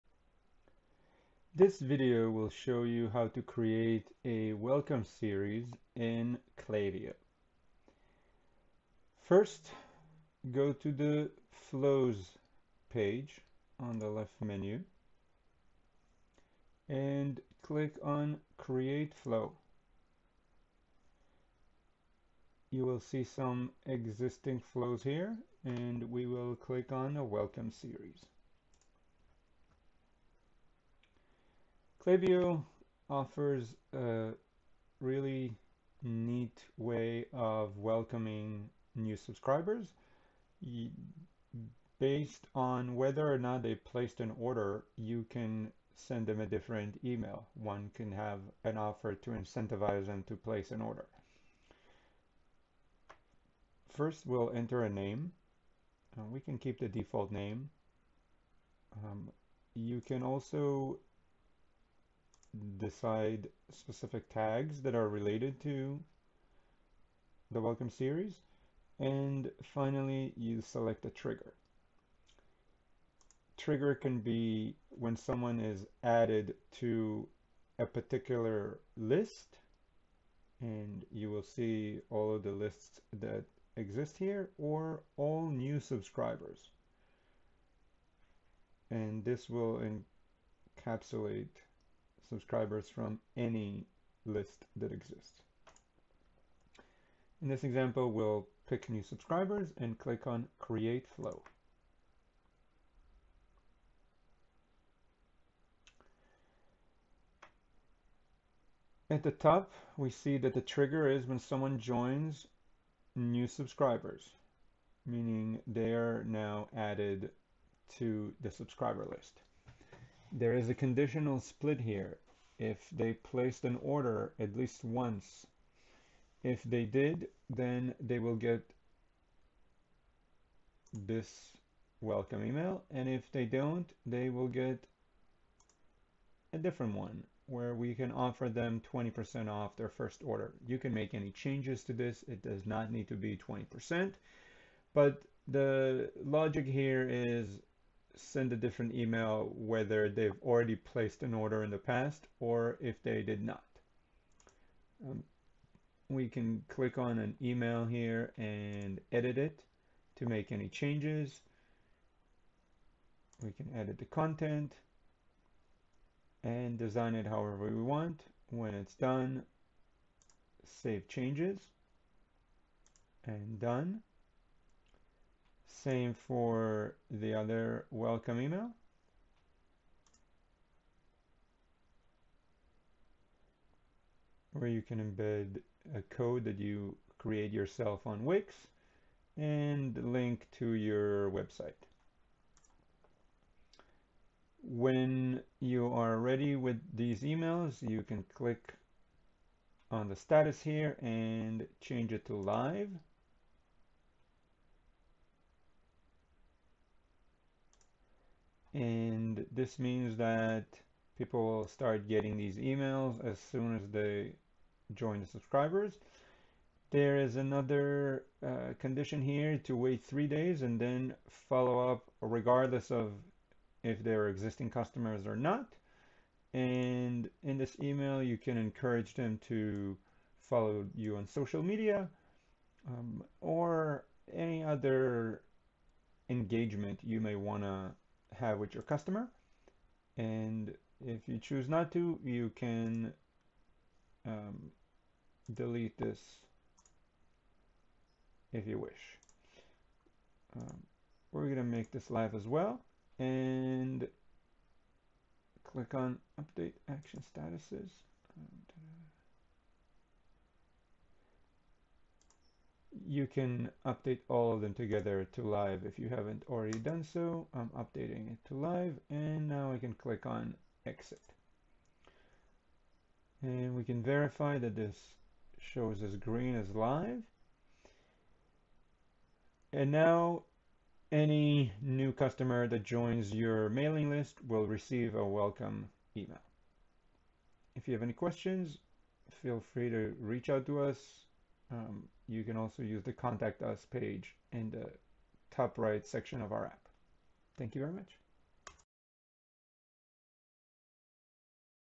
How to create an automated welcome email series - Wix Klaviyo Email Marketing App by PoCo
This video shows how to create a welcome email series in Klaviyo to be sent to new subscribers.

The Klaviyo app can be found on the Wix marketplace